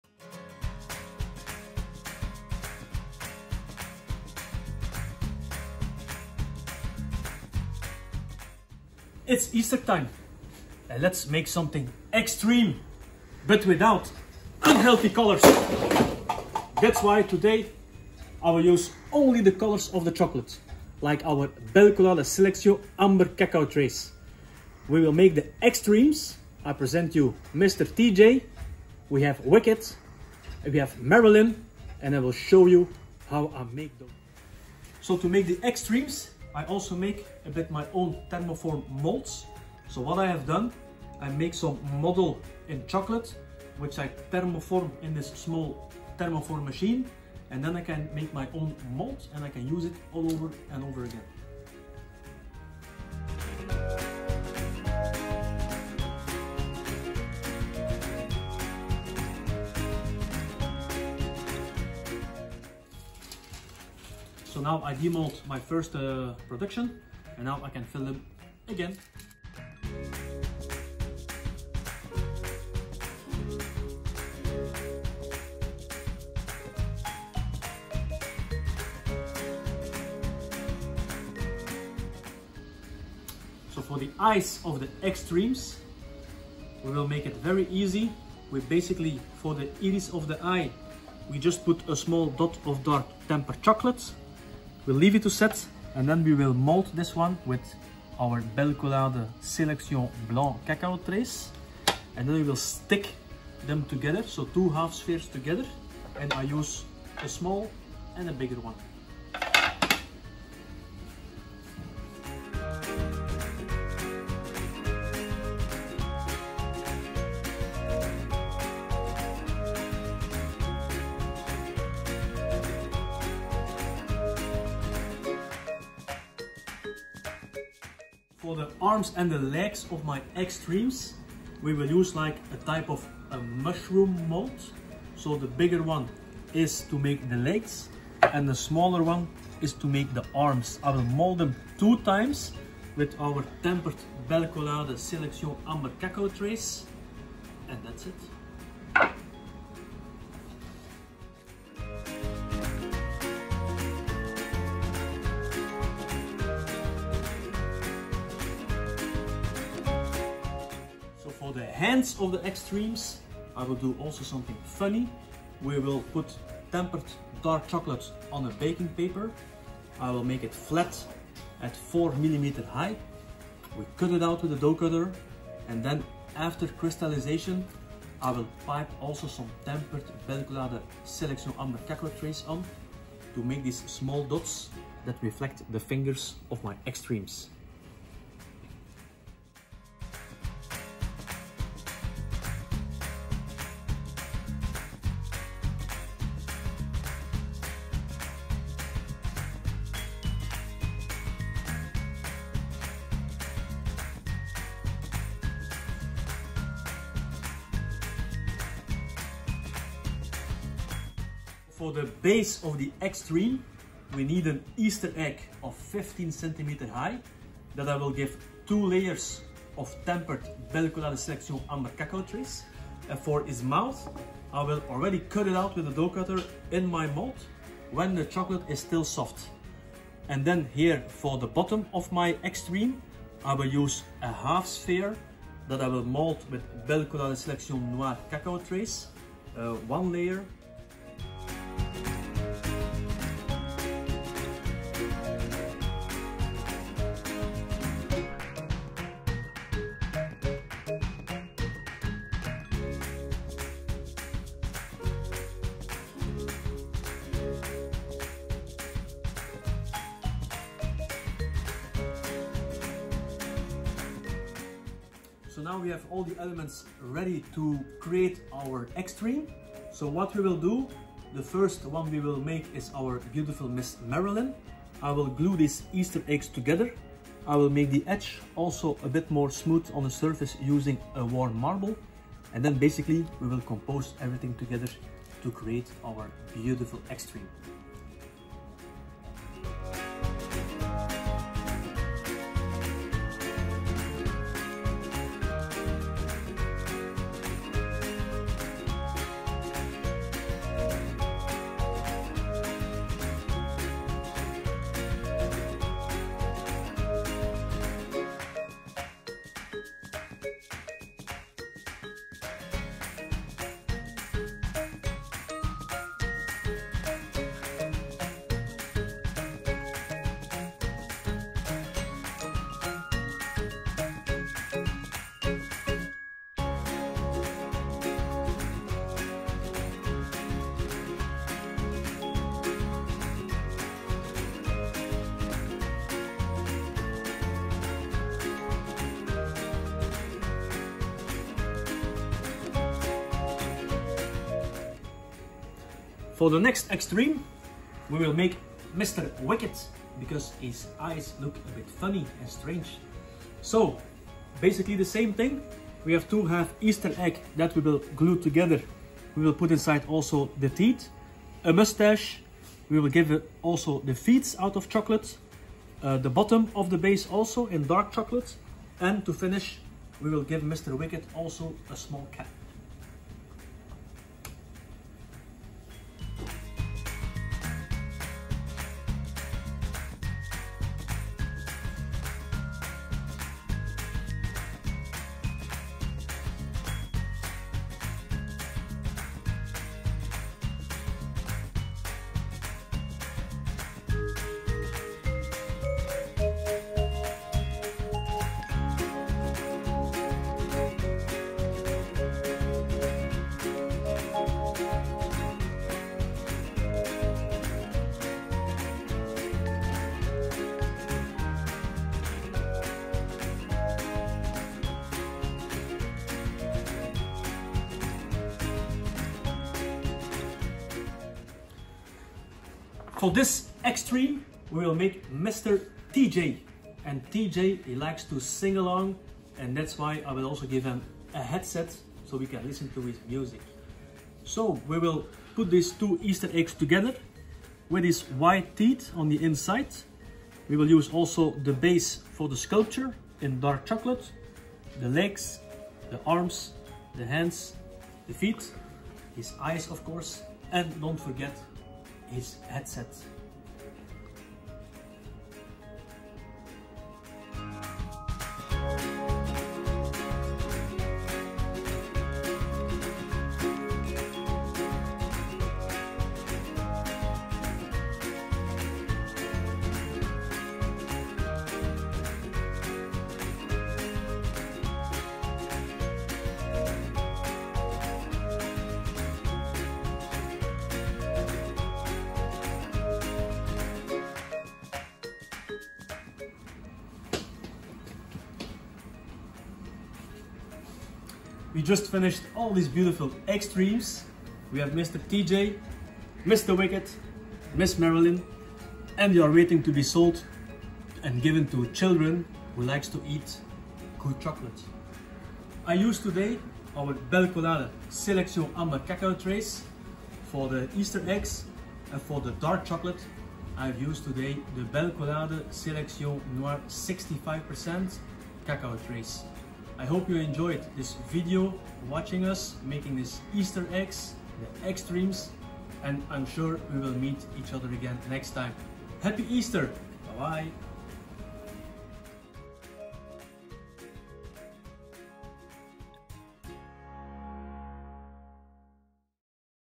Meet the EGGstreams
Bekijk de live demo van Michel Eyckerman en maak kennis met de EGGstreams.
Regardez la démonstration en direct de Michel Eyckerman et rencontrez les « EGGstreams ».